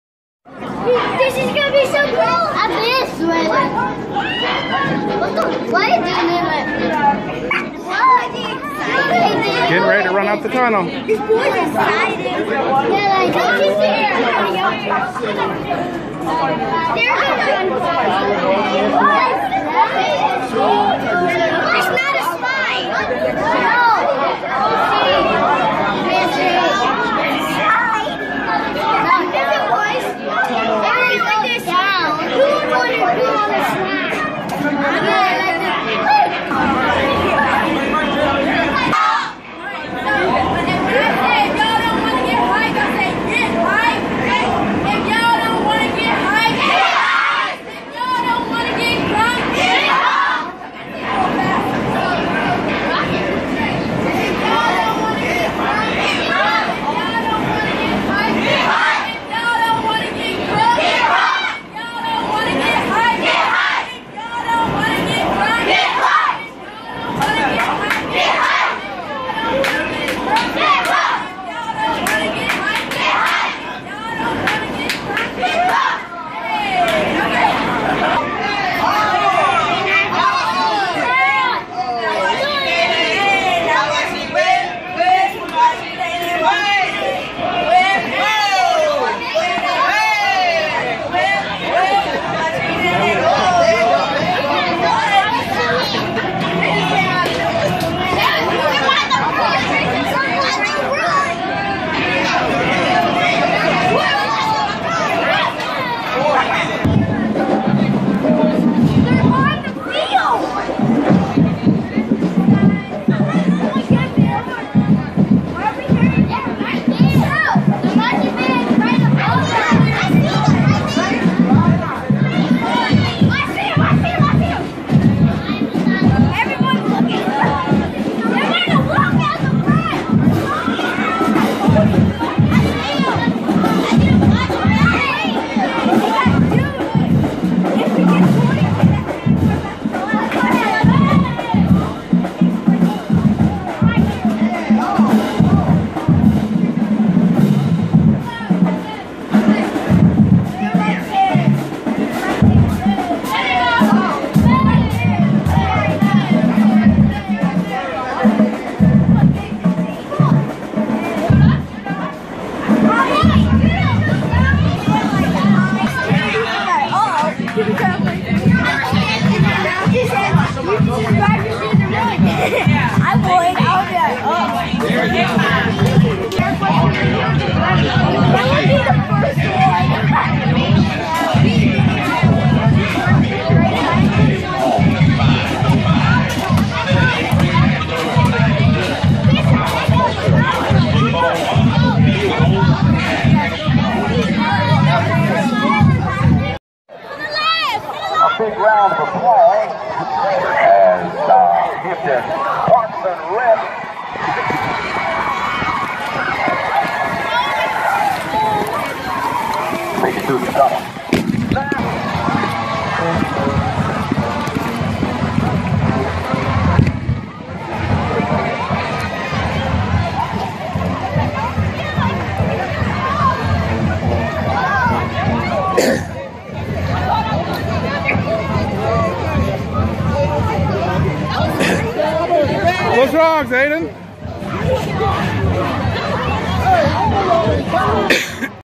Fox Hill Mighty Mites leading Hampton University Pirates out of the tunnel
Our youth football league got lead the Hampton University Pirates football team out of the tunnel at their home opener.